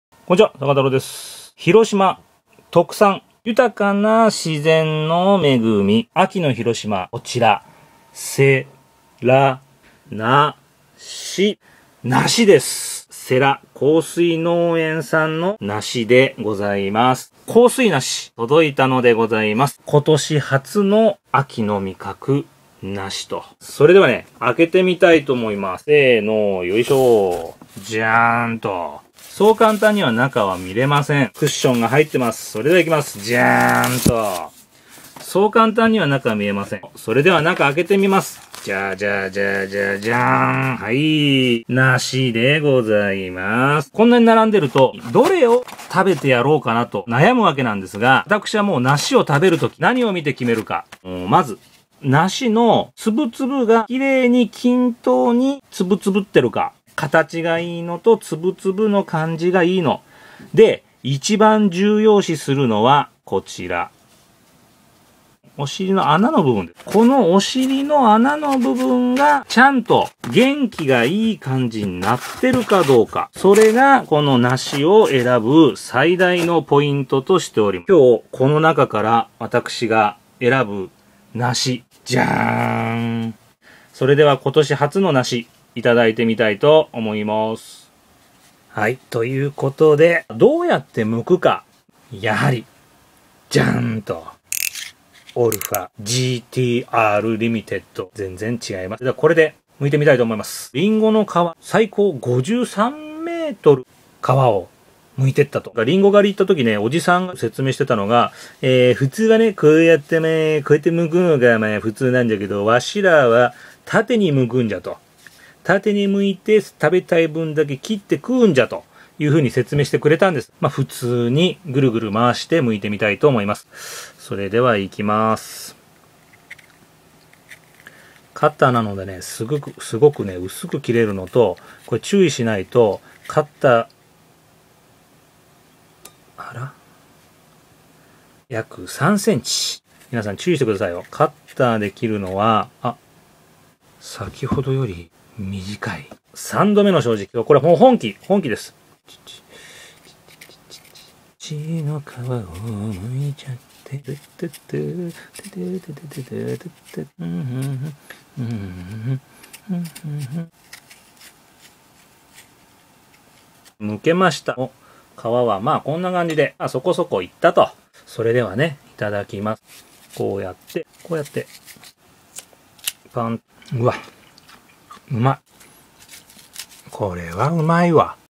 美味しい梨の見分け方~世羅幸水農園 ~ Japanese Pyrus pyrifolia~
広島県の世羅幸水農園・ビルネラーデンで収穫された幸水梨です。